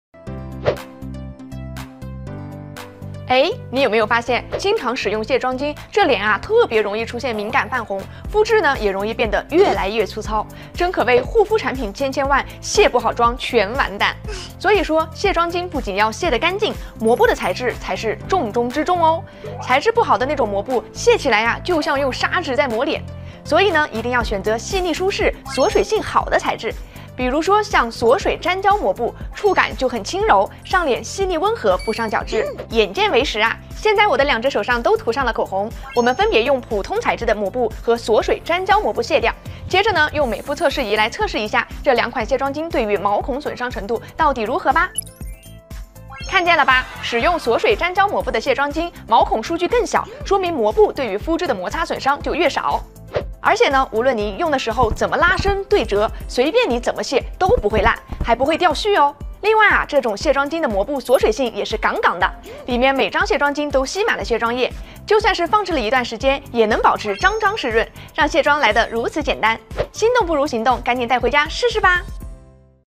第2集 想要解锁“既卸妆又养肤”的全新卸妆体验？雅姿净透水润卸妆巾带您实现~安利雅姿健康丽龄一分钟小课堂
雅姿净透水润卸妆巾的介绍:
🌿洁净保湿：深层清洁，可轻松有效地卸除持久和防水彩妆，清洁堵塞肌肤毛孔杂质，卸妆后肌肤清爽不油腻，保湿不紧绷；
🌿植萃呵护：蕴含地中海马耳他红石榴提取物等多种植萃成分的养肤卸妆巾，卸妆同时滋润肌肤；
🌿柔软亲肤：臻选亲肤膜布，锁水性好更水润，细腻柔软呵护娇嫩肌肤。

微信搜索公众号：沙迦笔记；
+vx：Bruce1934